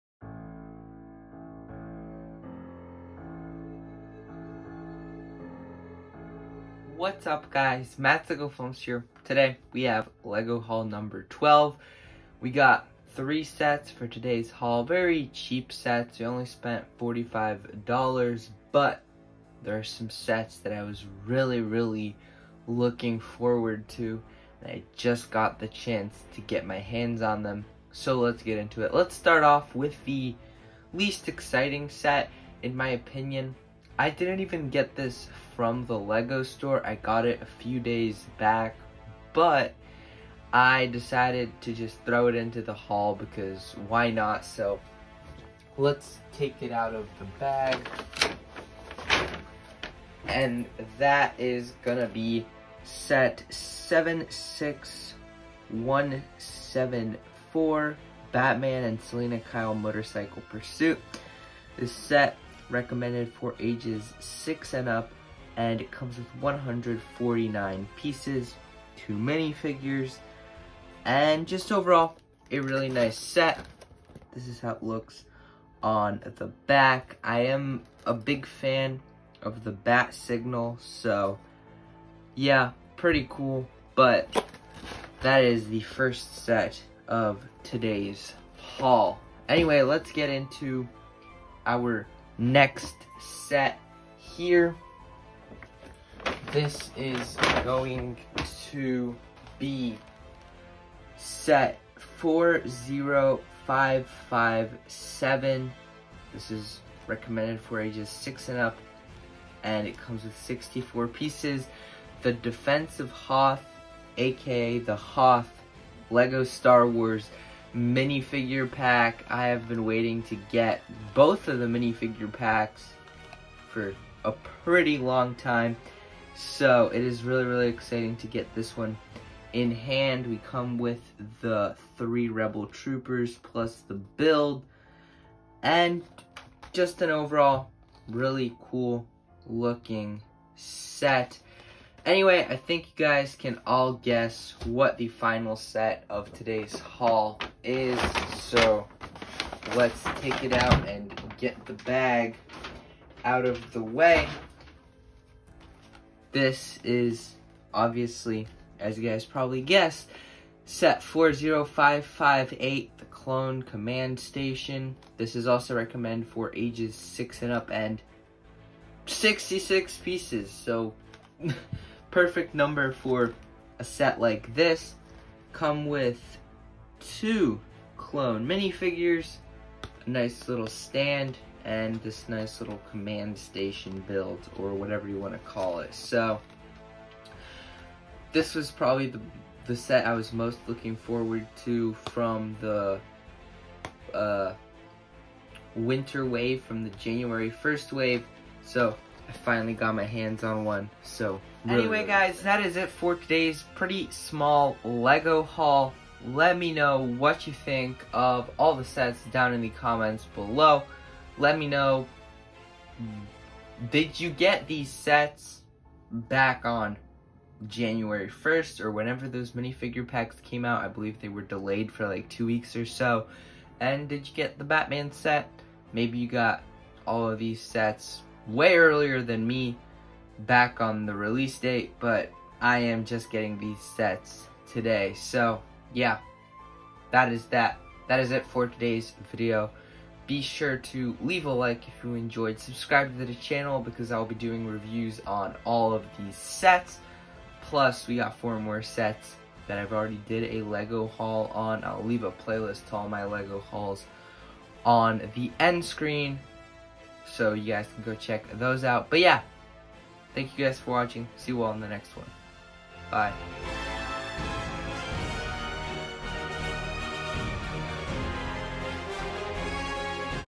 LEGO Haul #12 (Finally Got These Sets)
In today's video I bought more LEGO set's. I got three sets for today's haul. I will talk about the sets and share my thoughts on them.